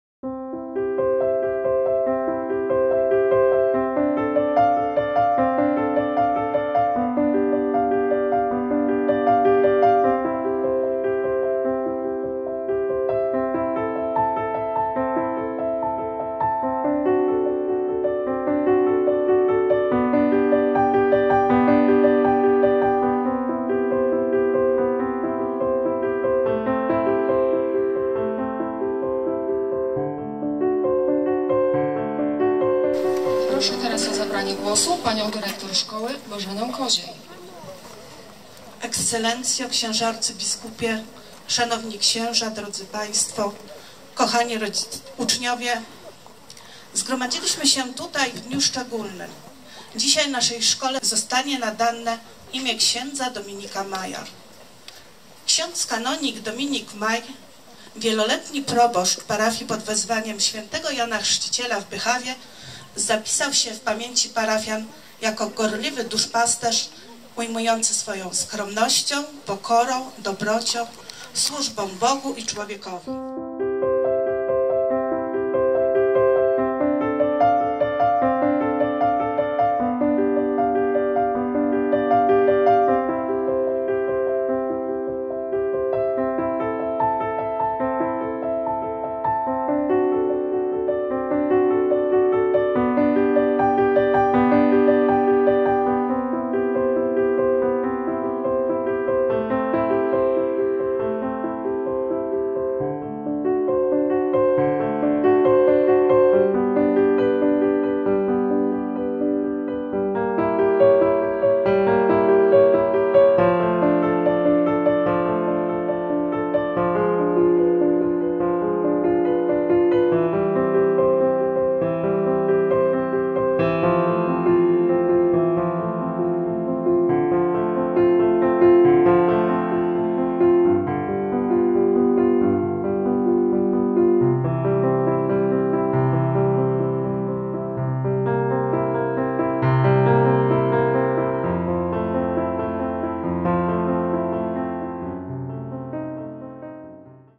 Nadanie SP w Zaraszowie imienia ks. Dominika Maja cz. 2
16 września 2018 r. odbyła się uroczystość nadania Szkole Podstawowej w Zaraszowie imienia ks. Dominika Maja.